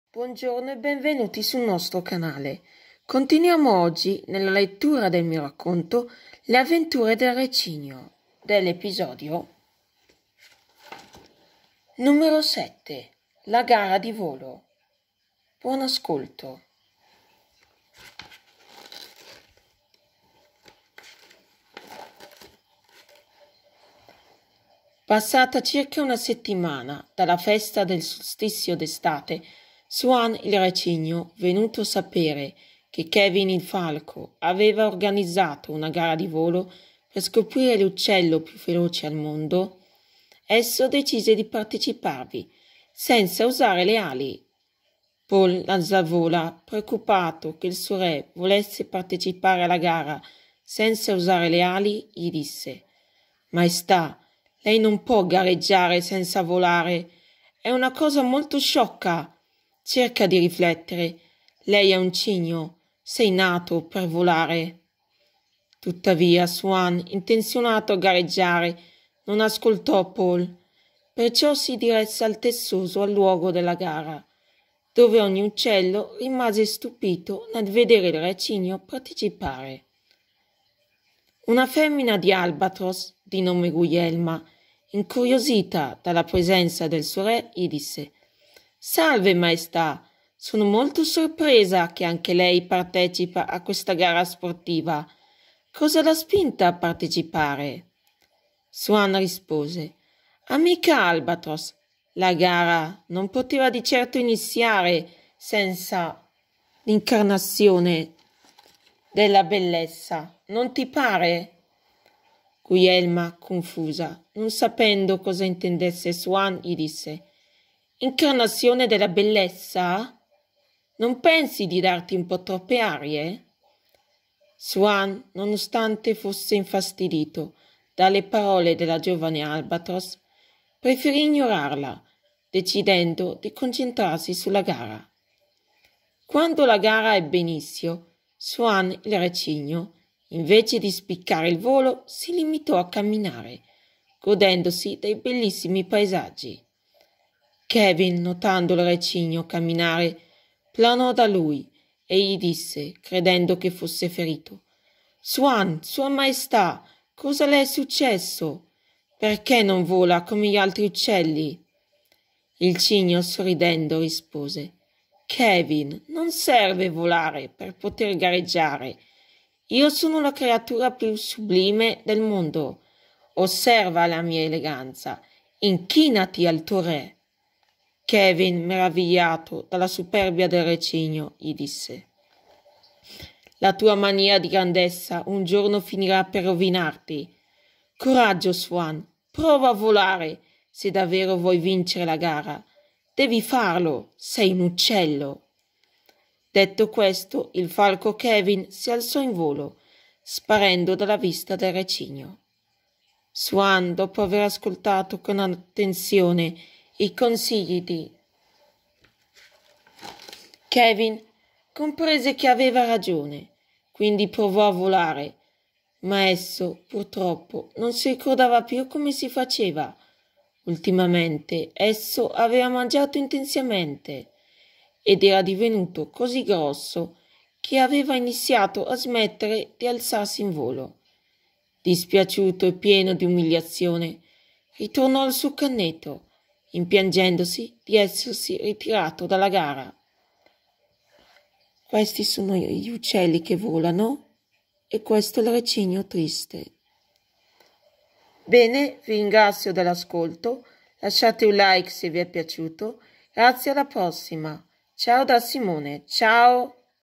Le avventure del re cigno ep.n.7 La gara di volo 🦅🦢🦆
racconto realizzato da Simone con disegni unici e originali